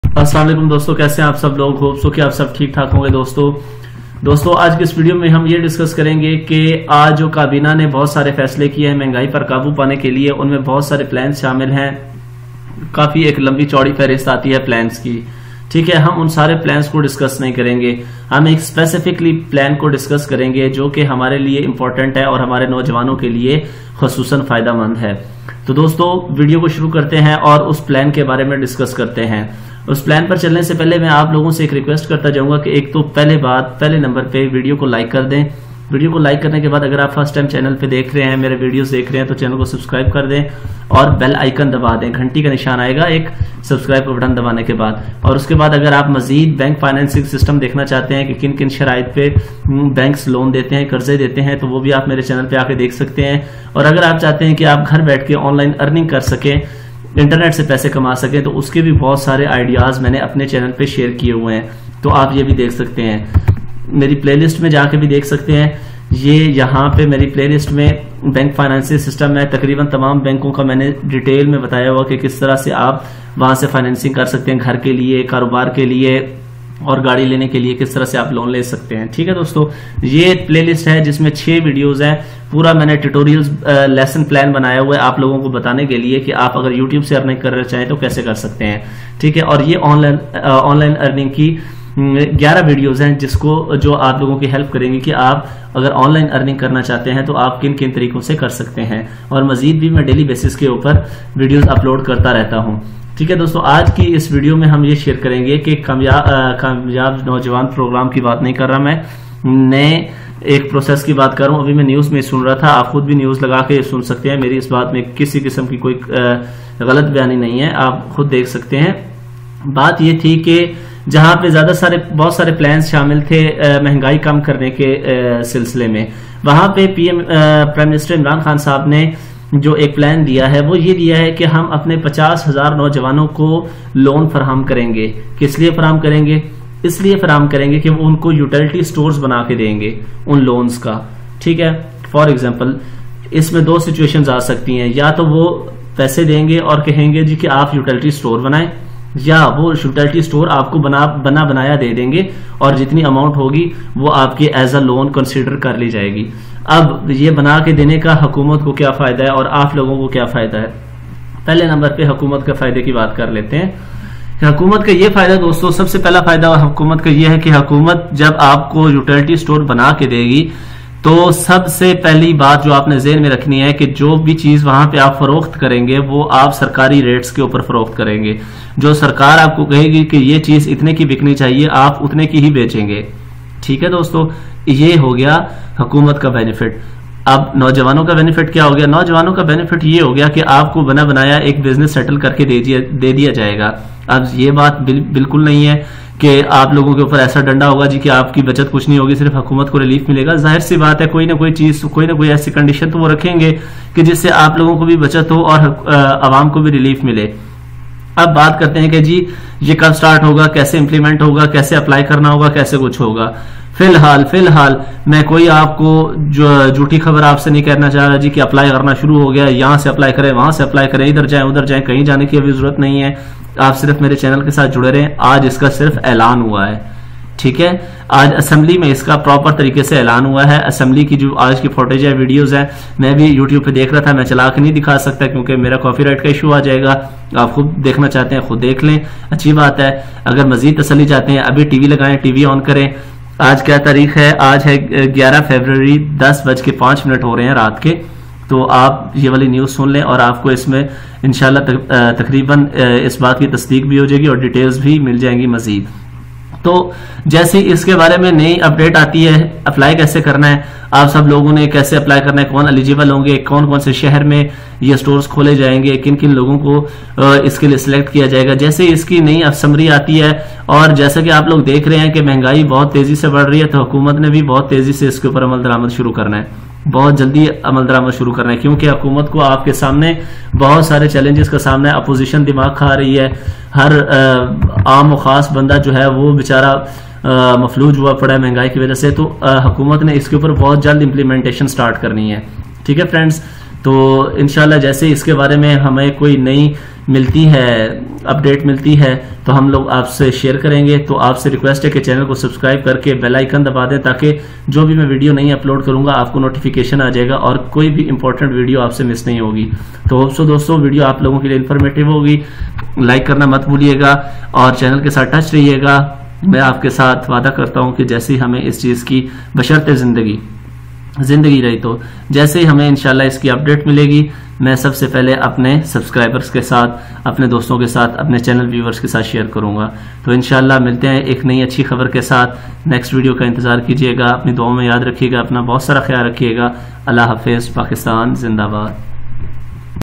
Pm Imran khan announced loan for youth to build utility stores to overcome poverty
imran khan in today speech decided with his cabinet to give loan to youth for opening utility store in order to reduce the market product prices in all over pakistan.